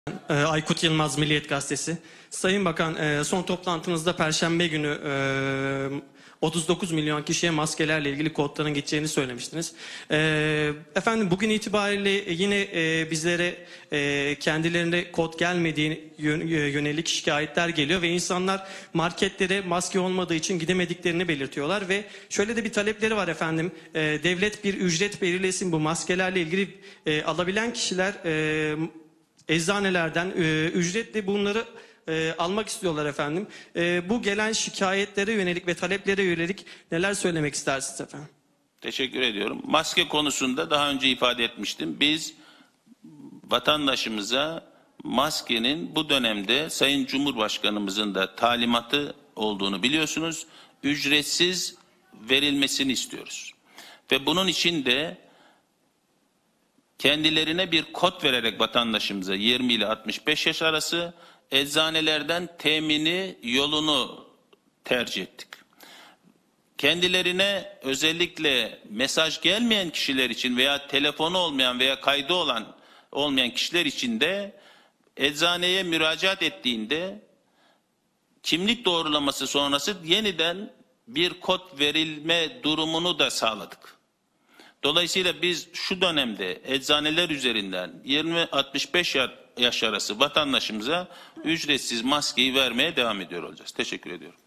Maske için telefonlarına kod gelmeyenler ne yapacak? Sağlık Bakanı Fahrettin Koca açıkladı
Bazı vatandaşların maske için başvuru yapmasına rağmen SMS'leri gelmediği için maske temin edemedikleri yönündeki şikayetleri değerlendiren Sağlık Bakanı Fahrettin Koca çözüm yöntemini açıkladı. İşte SMS'niz gelmediyse yapmanız gerekenler.

tv100 uydu bilgileri:

TÜRKSAT 4A
Frekans: 12729
Sembol: 30000
Polarizasyon: V (Dikey)